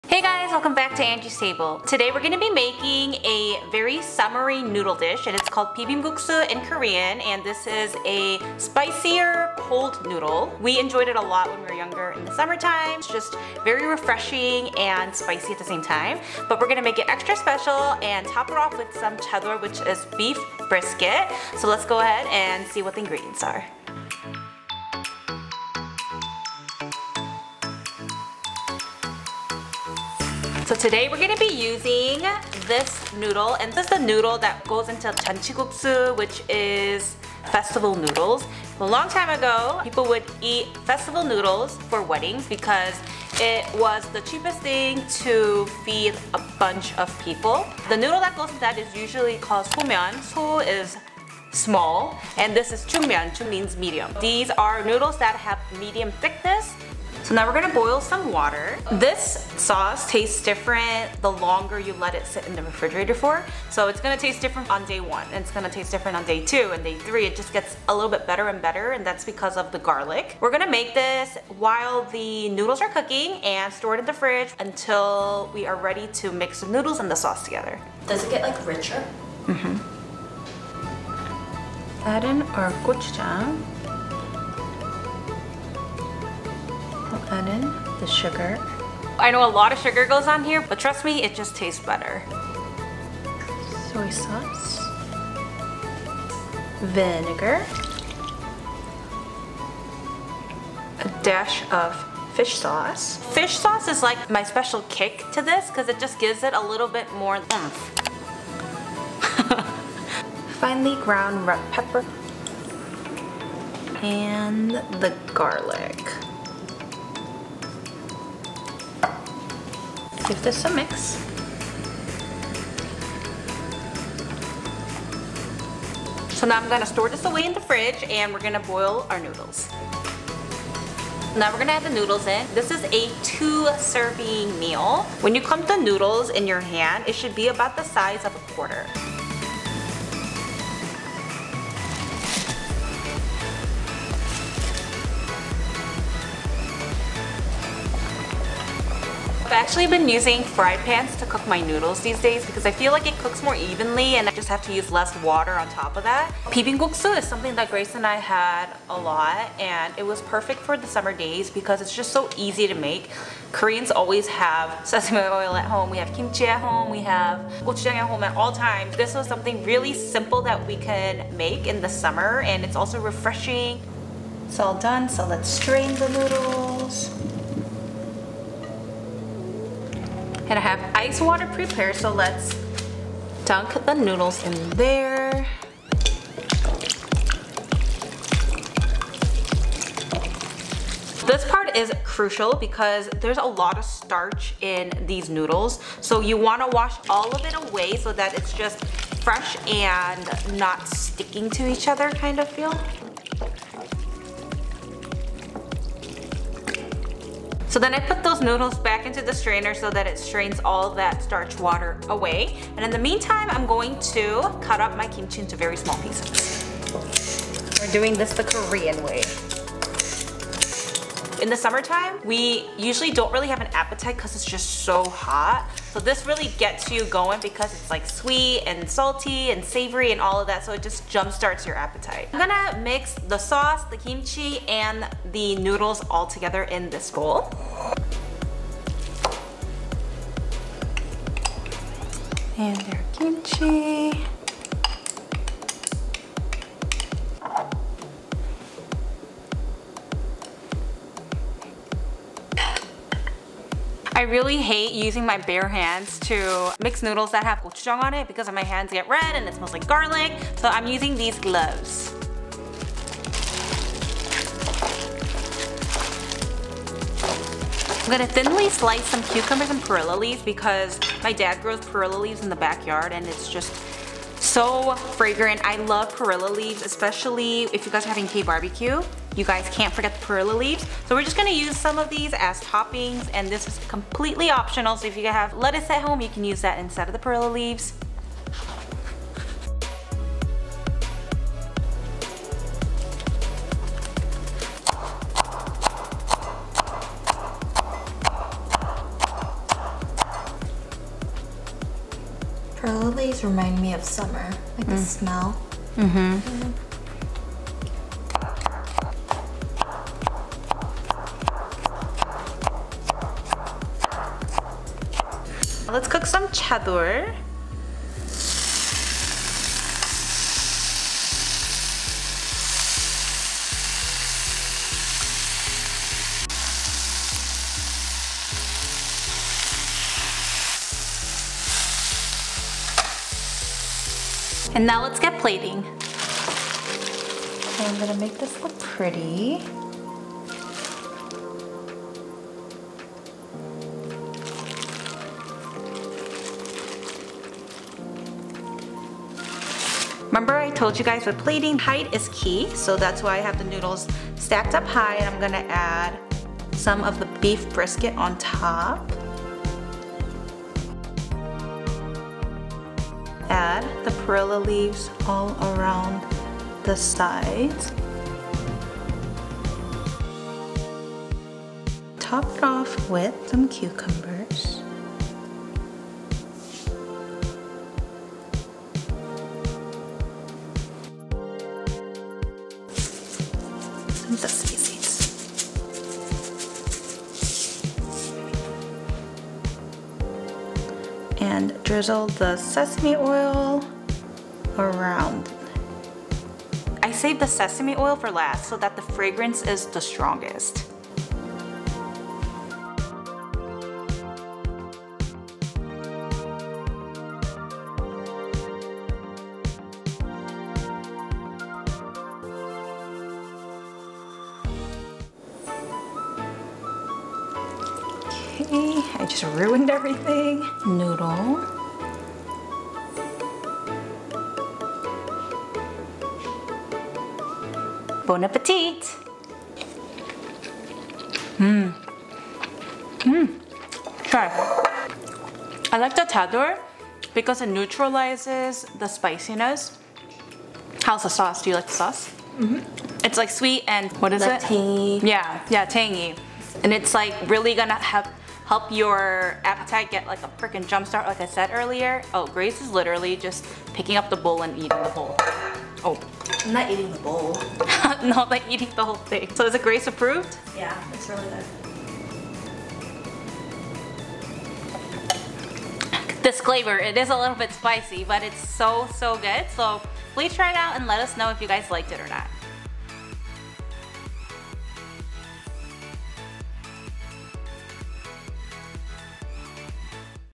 Korean Beef Brisket Spicy Mixed Cold Noodles // 차돌 비빔국수 (Summer Recipes)
Hey guys! Hope you’re ready for another summer recipe! Today, we’ll be showing you how to make a very popular Korean summer dish called bibim guksu, which is a spicy mixed cold noodle. Instead of the traditional egg and cucumber on top, we will be showing you how to enjoy it with a KBBQ twist using thin beef brisket and perilla leaves. This is so so delicious and literally perfect for those days that you just don’t have a craving for anything (it happens more frequently in the summer time for us).

Ingredients:
1/3 Cup Gochujang
4 Tbsp Vinegar
3 Tbsp Sugar
1/2 Tbsp Fine Red Pepper Powder
1/2 Tbsp Garlic, minced
1 Tbsp Soy sauce
1/2 tsp Fsh sauce
1/4 Cup Kimchi,chopped
2 tsp Sesame oil
2 servings of Wheat noodles, medium size (2 quarter size bundles)
1 Persian cucumber
10 Perilla leaves
10-15 Pieces of Chadol (thinly sliced beef brisket)

Directions:
1. In a bowl, add gochujang, sugar, soy sauce, vinegar, fish sauce, fine red pepper powder, and minced garlic. Mix until well combined. Cover and set aside in refrigerator until ready to use.
2. Boil a pan of water. Add noodles once water comes to a boil. Cook until soft.
3. Strain the noodles in a colander, and place in a cold ice water bath.
4. Make sure to wash the noodles and get all of the starch out. If not washed properly, it will have an unpleasant smell. When complete, strain and set noodles in a larger bowl.
5. Chop kimchi into small pieces. Set aside. Try using scissors, it’s faster!
6. To the bowl with the noodles, add the sauce we prepared earlier and mix well.
7. Heat a pan on medium to high heat. Cook the brisket and set aside.
8. Prepare the perilla leaves for garnishing by chopping into smaller strips. This step is optional, and you may replace with lettuce.
9. Cut cucumbers thinly and set aside.
10. Plate noodles and top with brisket and cucumbers and perilla leaves.
11. Serve, and enjoy!